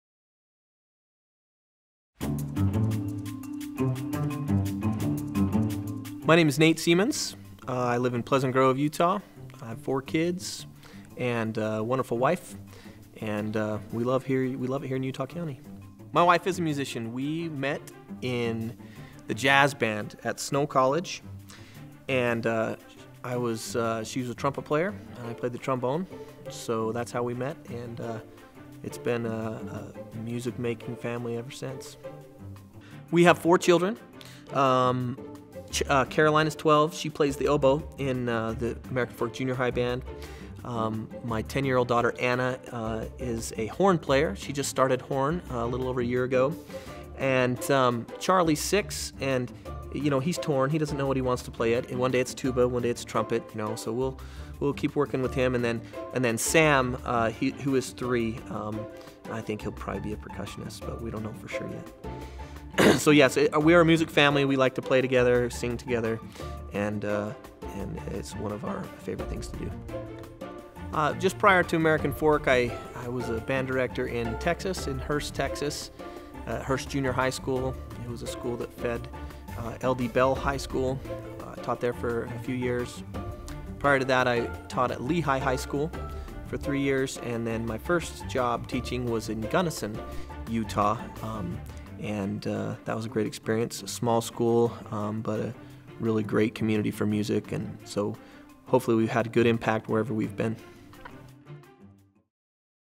Introducing Nate Seamons
Nate Seamons was introduced as the incoming Director of Bands at American Fork High School on March 14, 2016. He replaces John Miller, who is retiring after 30 years at American Fork.

In this video segment, Mr. Seamons discusses his personal life and previous experience in teaching music.